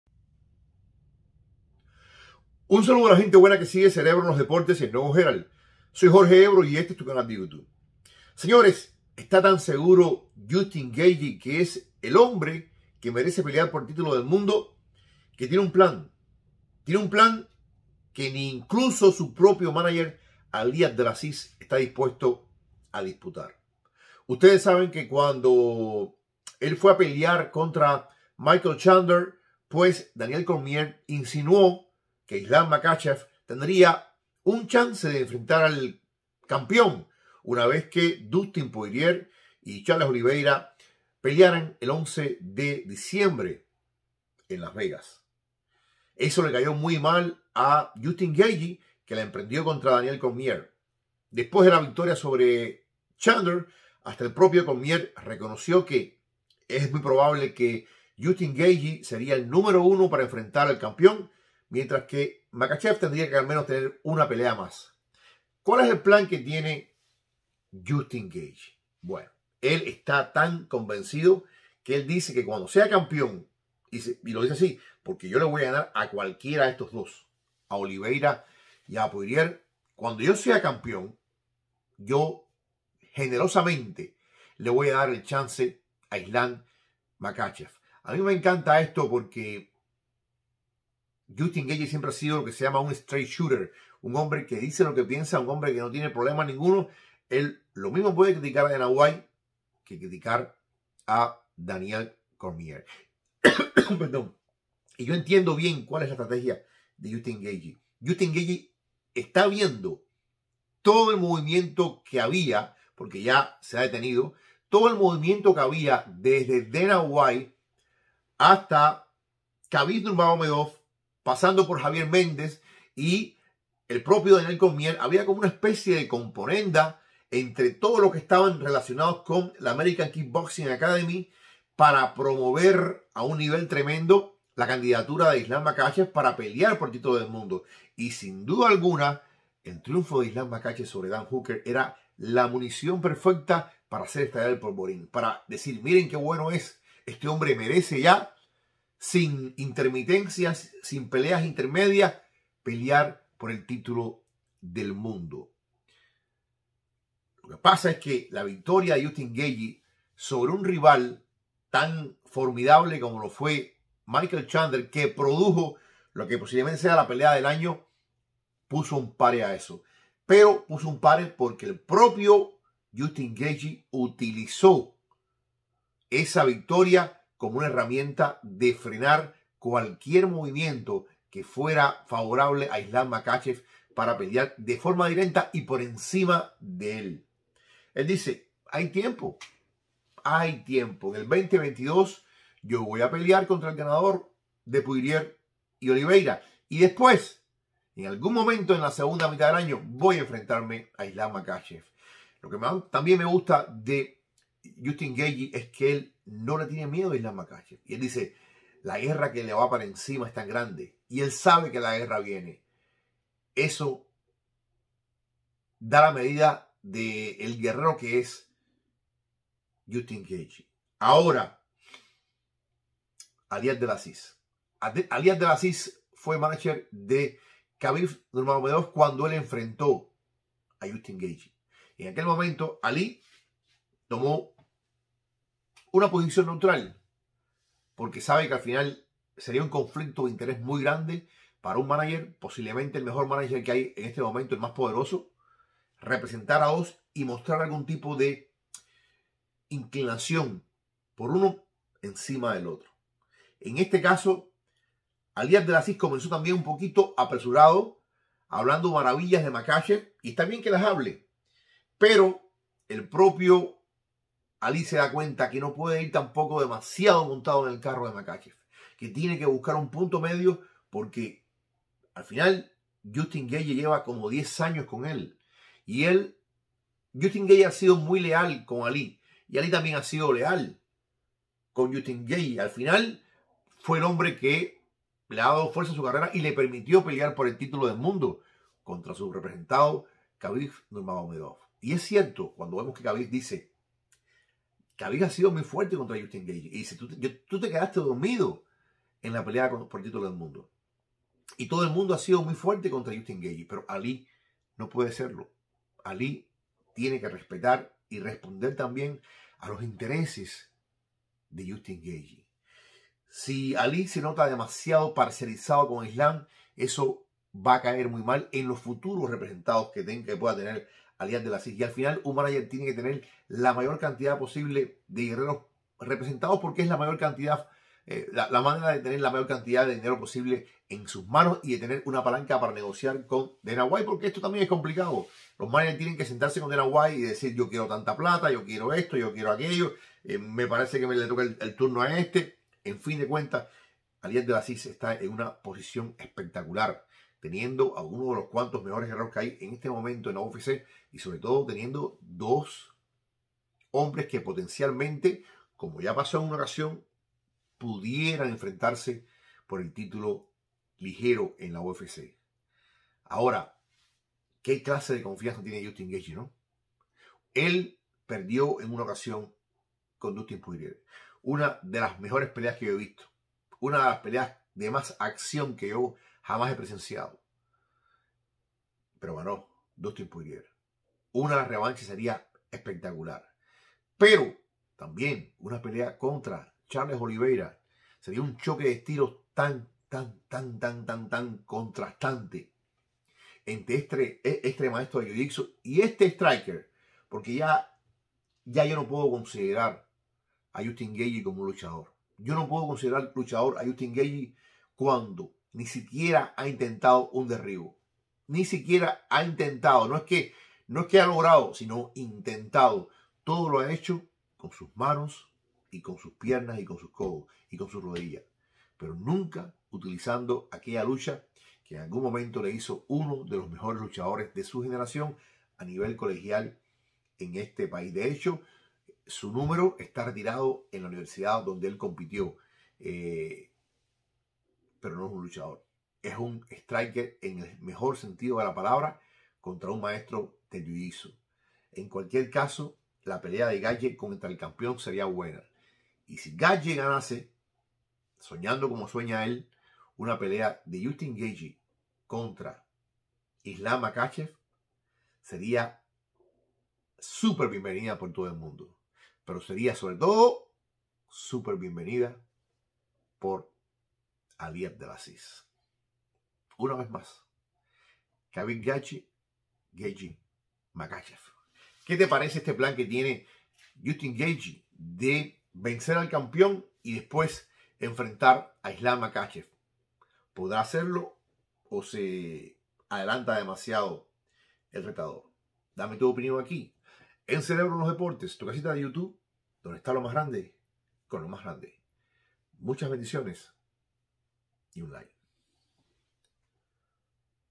JUSTIN GAETHJE a Makhachev, la guerra viene y tú lo sabes
El luchador de UFC Justin Gaethje reveló su plan para ganar el título de peso ligero de UFC y luego hacer su primera defensa del título contra Islam Makhachev.
Gaethje no estaba contento cuando el comentarista de UFC Daniel Cormier sugirió que Makhachev debería ser el próximo en la fila para pelear por el título de peso ligero después de vencer a Dan Hooker en UFC 267. En lo que respecta a Gaethje, claramente es el siguiente hombre en la fila por el cinturón después venciendo a Michael Chandler en UFC 268. Entonces, aunque sabe que Makhachev es un gran peleador por derecho propio que merece pelear por el cinturón, Gaethje confía en que debería tener la próxima oportunidad por el título.
Sin embargo, Gaethje también sabe que Makhachev quiere desesperadamente la oportunidad por el título, por lo que ha ideado un gran plan. Hablando con MMAFighting.com, Gaethje sugirió un escenario de imagen de título de peso ligero en el que gana el cinturón del ganador del evento principal de UFC 269, y luego hace su primera defensa del título contra Makhachev. Eche un vistazo al plan de Gaethje a continuación.
"Lo dejaré ir primero si consigue su lugar. Derrotaré a uno de estos tipos (Charles Oliveira o Dustin Poirier) y luego le permitiré ir primero. Joder, sí, estoy deseando que llegue eso ", dijo Gaethje. "La guerra se acerca. Tiene que saber una cosa, se acerca una maldita guerra. Así que tráelo ".